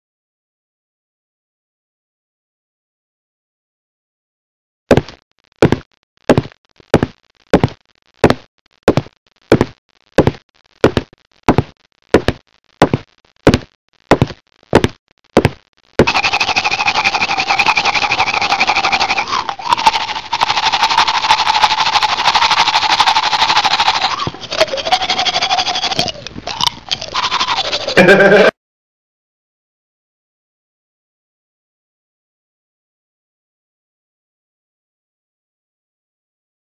Cookiehero
En film gjord av Fabian och Isak på kvällskursen Hjältar och Hjältinnor på KomTek Järfälla.